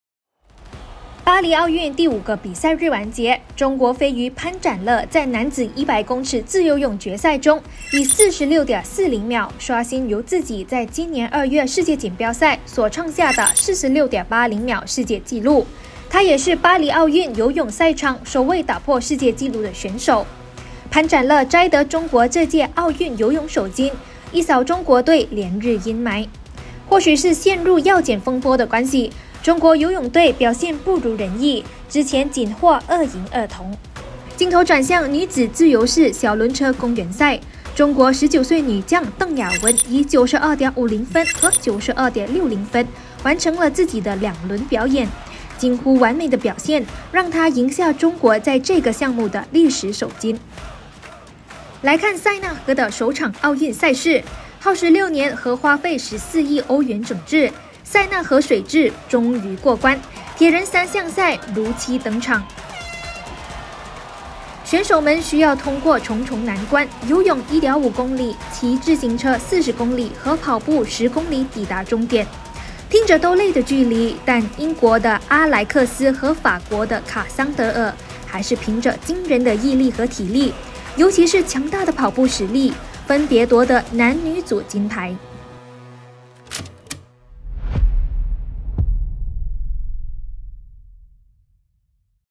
【巴黎奥运】中国泳队首金 潘展乐自由泳破世界纪录
中国飞鱼潘展乐在男子100公尺自由泳决赛中，以46.40秒刷新由自己在今年2月世界锦标赛，所创下的46.80秒世界纪录。他为中国摘下这届奥运游泳首金，也成为巴黎奥运游泳赛场，首位打破世界纪录的选手。